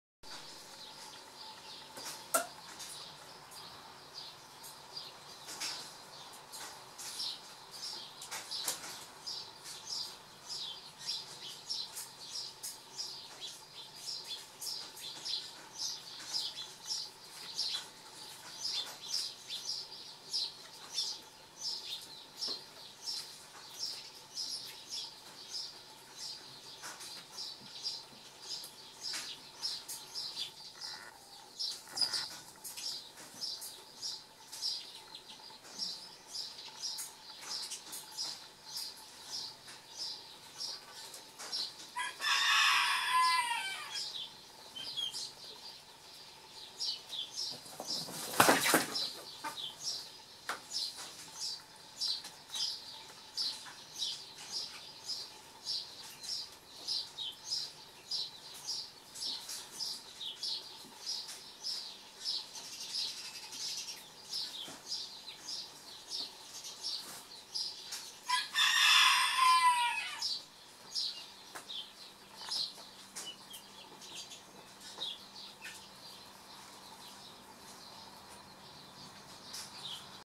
Ruby Laying her Egg - Silver-duckwing hen - HD
Watch Rudy our little Silver Duckwing lay her egg, so much effort goes into laying. AndreaEL Photography Southland New Zealand